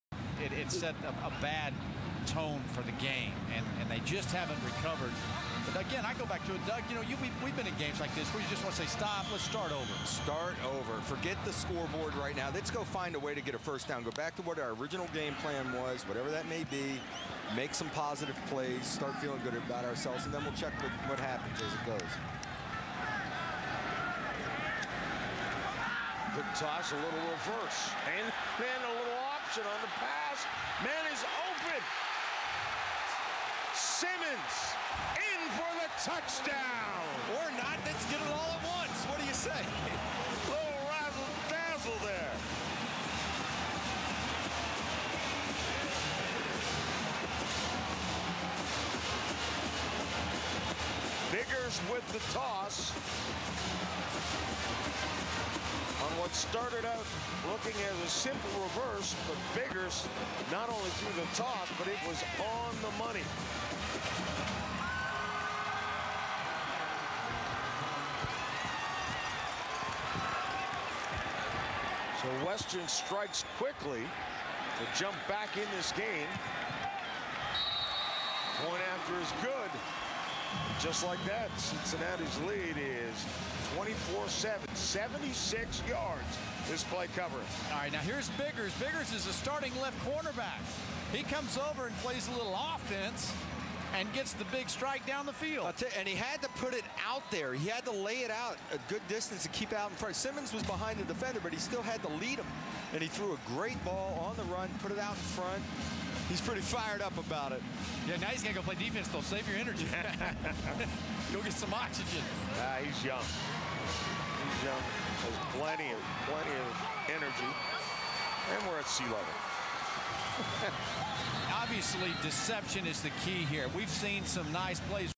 International Bowl 2007
E J Biggers reverses on a hand-off from freshman running back Glenis Thompson, complete to Jamarko Simmons about 50 yards downfield. Simmons does the rest, outrunning the defense and tiptoeing the sideline for a 76-yard score
But in the end it was Cincinnati Bearcats 27, WMU Broncos 24